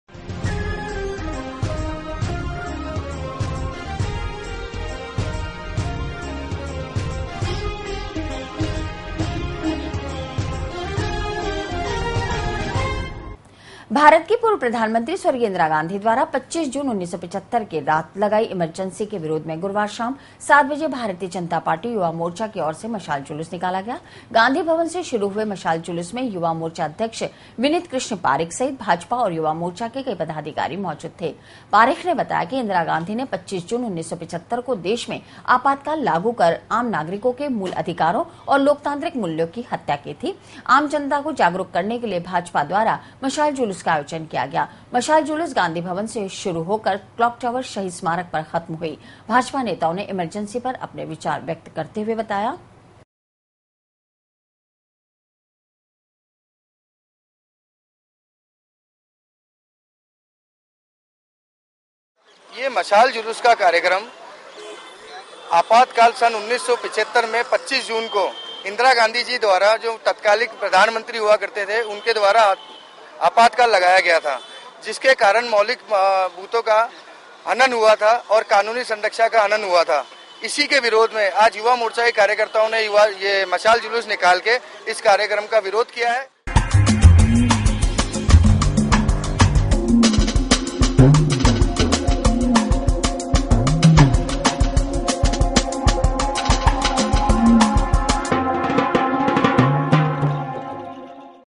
Emergency Ke 40 Sal Pure. इमरजेंसी के 40 साल पूरे विरोध में निकाला मशाल जलूस।
तत्कालीन प्रधानमंत्री इंदिरा गांधी द्वारा लगाई गई इमरजेंसी के 40 साल पूरे। भारतीय जनता युवा मोर्चा ने विरोध में निकाला मशाल जलूस।